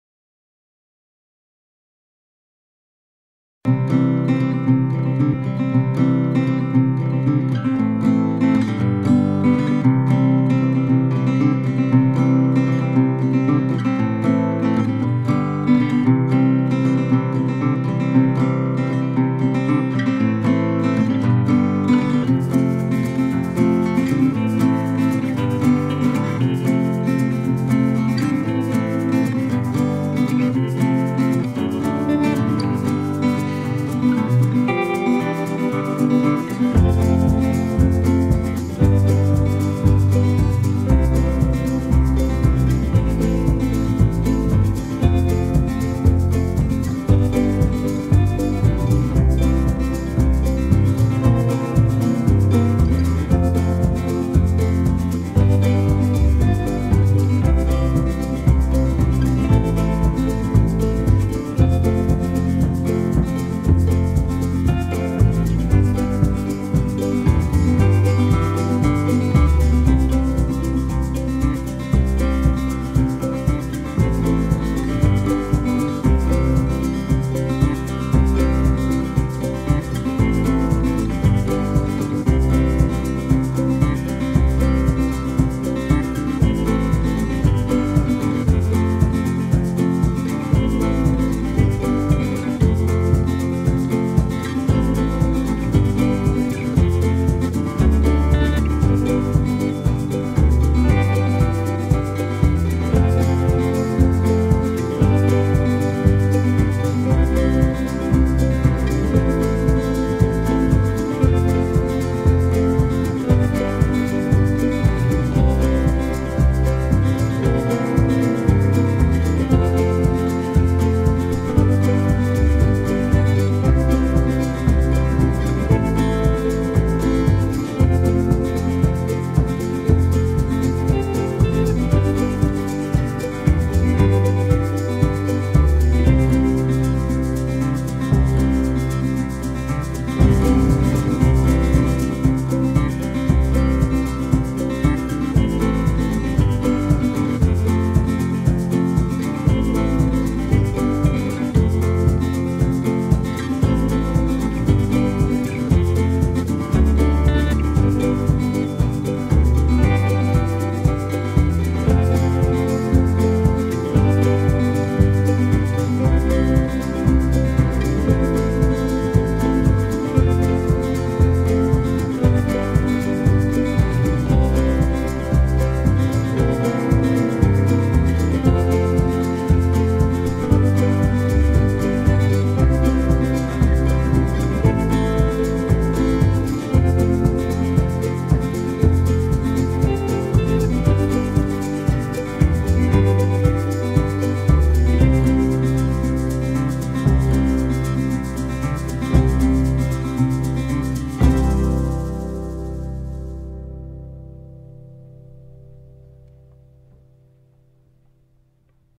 Вожская засека 2023
Велопоходик одного дня по местам оборонительных сооружений Вожской засеки. Август 2023